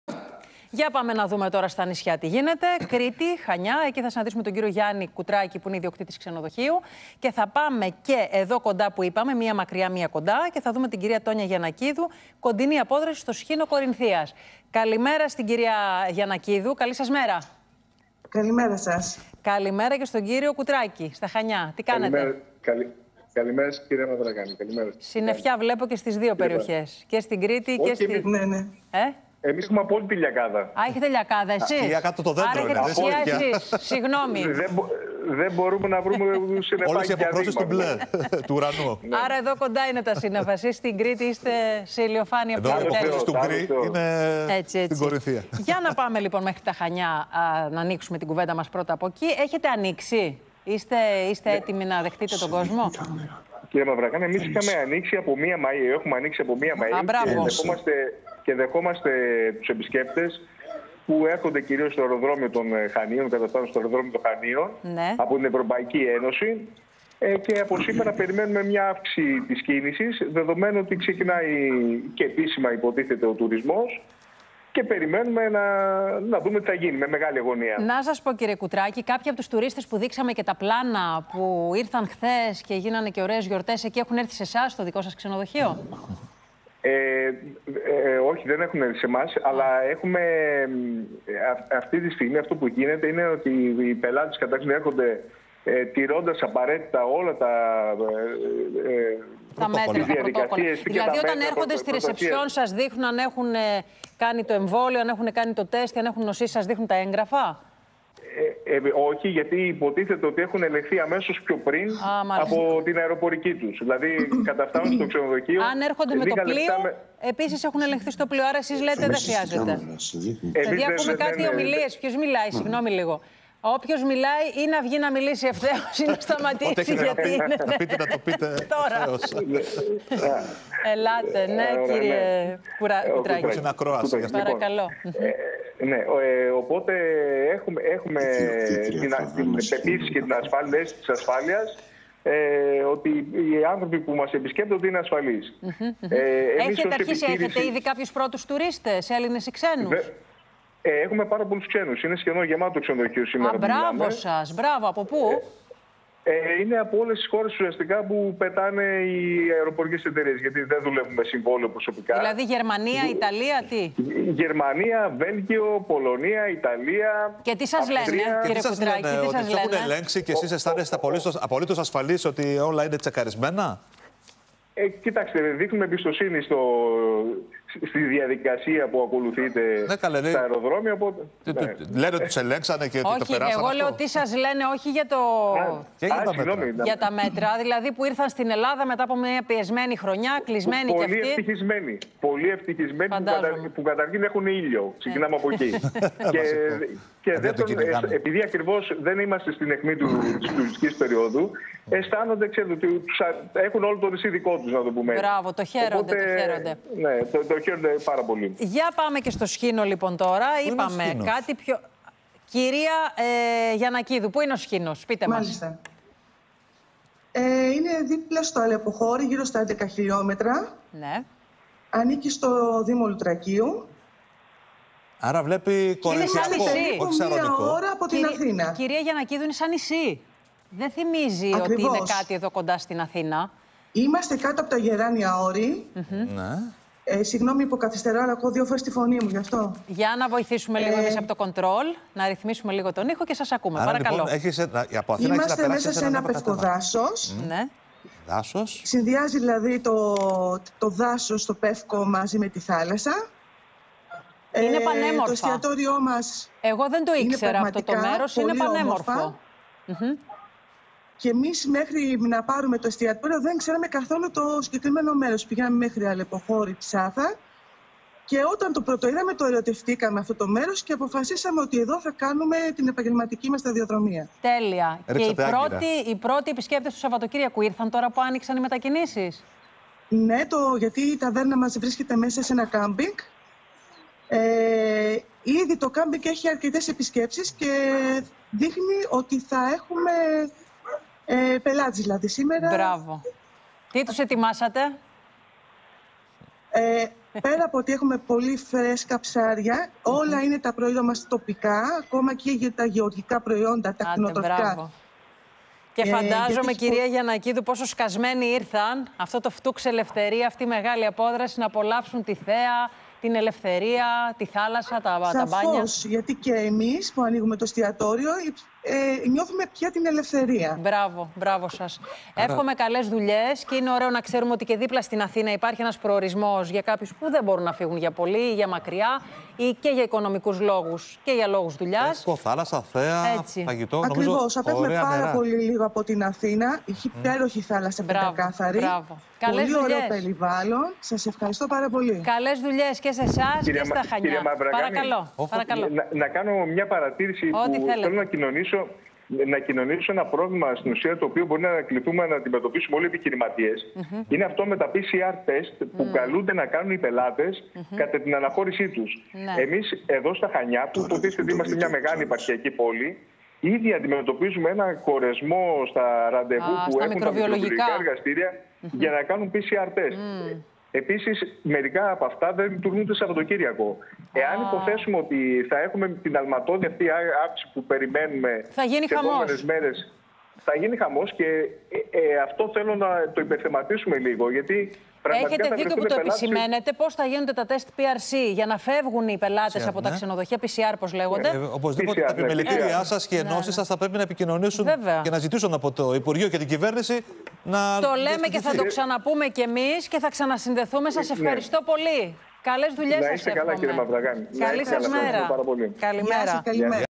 TO ANOIΓΜΑ ΤΟΥ ΤΟΥΡΙΣΜΟΥ - ΣΥΝΔΕΣΕΙΣ ΜΕ ΧΑΝΙΑ ΚΑΙ ΣΧΙΝΟ
ΠΩΣ ΘΑ ΚΑΝΟΥΜΕ ΦΕΤΟΣ ΔΙΑΚΟΠΕΣ - ΤΙ ΘΑ ΙΣΧΥΕΙ ΣΤΑ ΞΕΝΟΔΟΧΕΙΑ

Σύνδεση με τα πανέμορφα Χανιά και τον ιδιοκτήτη ξενοδοχείου, ΓΙΑΝΝΗ ΚΟΥΤΡΑΚΗ

Σύνδεση με τον Σχίνο Κορινθίας και την ιδιοκτήτρια ταβέρνας, ΤΟΝΙΑ ΓΙΑΝΝΑΚΙΔΟΥ